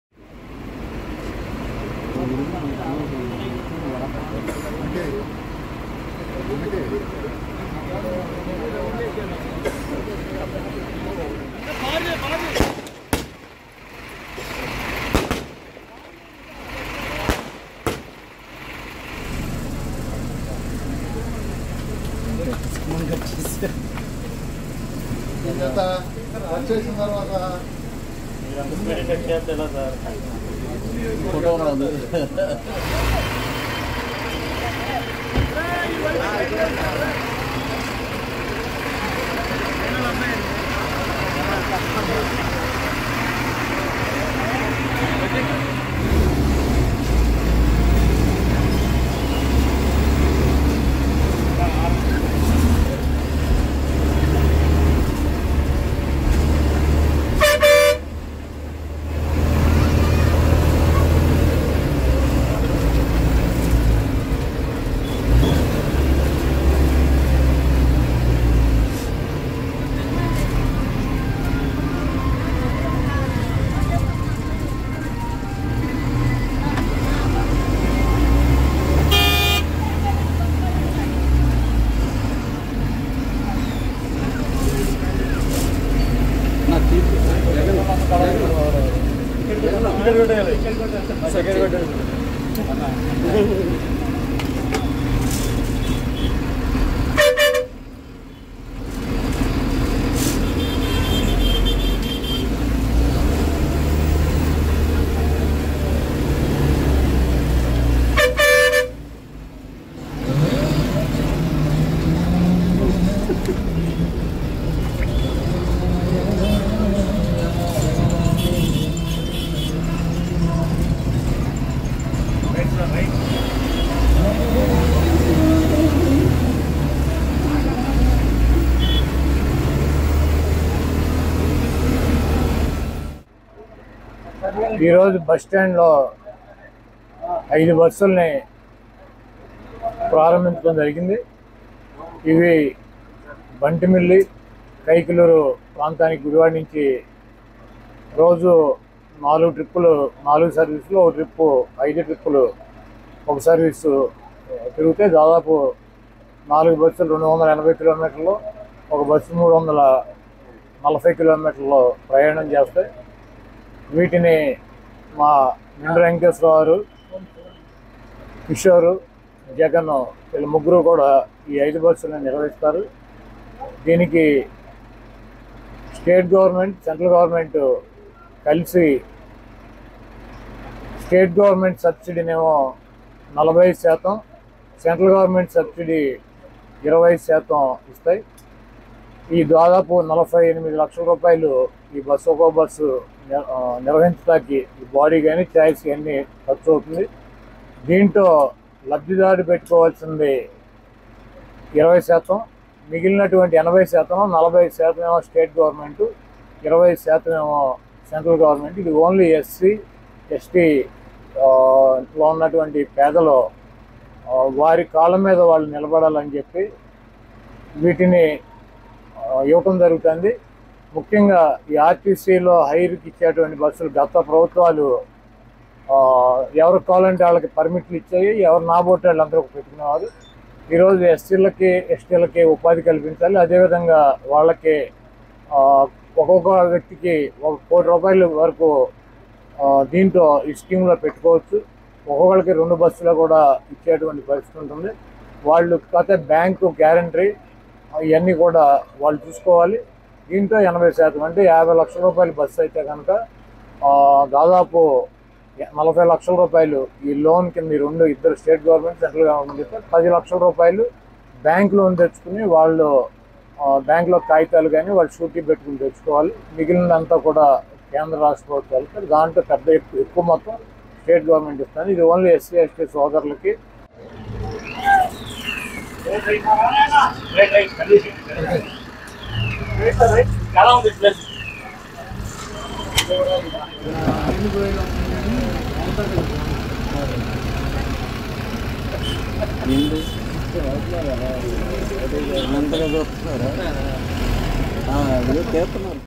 Kodali Nani Turns As APSRTC Bus Driver | Kodali Nani Bus Driving Video | AP News | Mobi Reporters
Watch : Kodali Nani Turns As APSRTC Bus Driver | Kodali Nani Bus Driving Video | AP News | Mobi Reporters